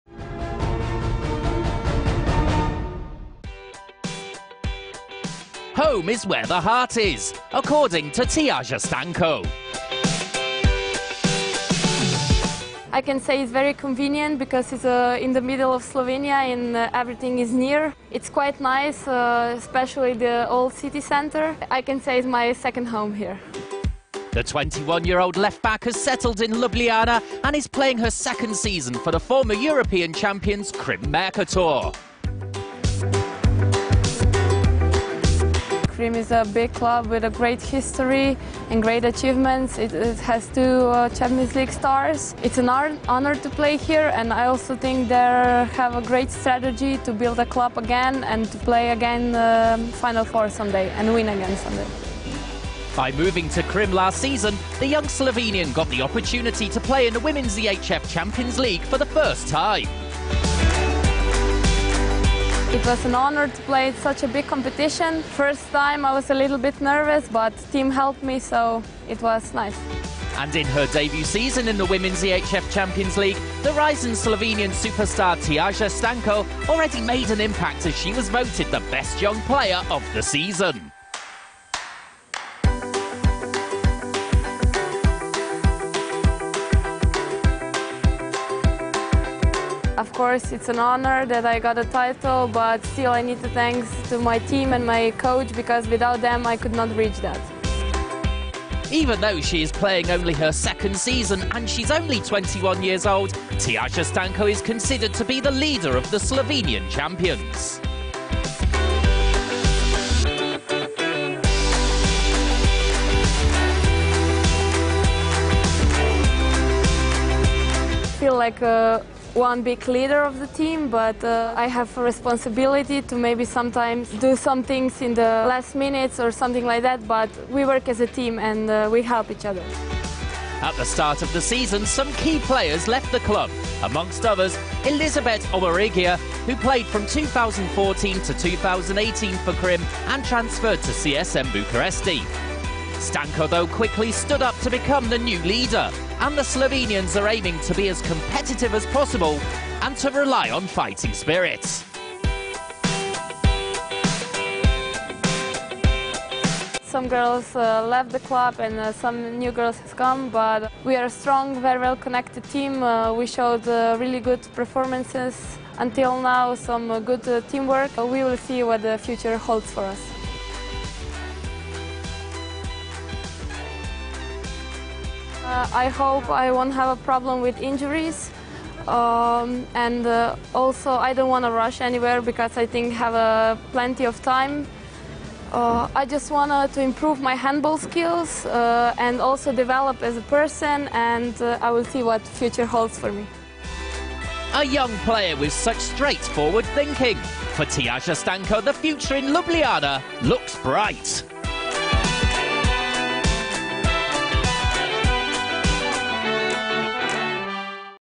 Closer Look | Round 5 | Women's EHF Champions League 2018/19
We met the young leader of the new Krim pack - Tjasa Stanko.